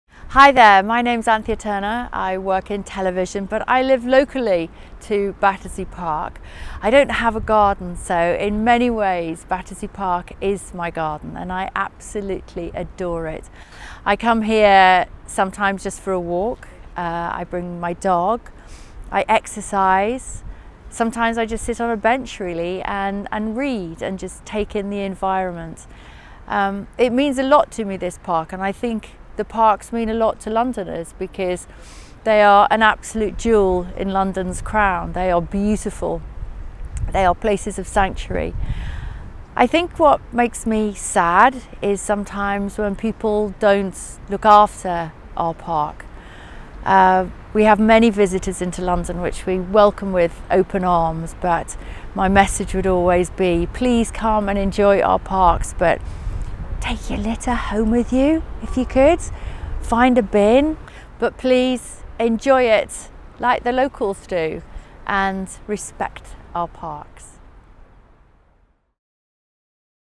Anthea Turner on Battersea Park
The TV presenter helps us launch our summer Love Parks campaign by telling us what she loves about her favourite Wandsworth park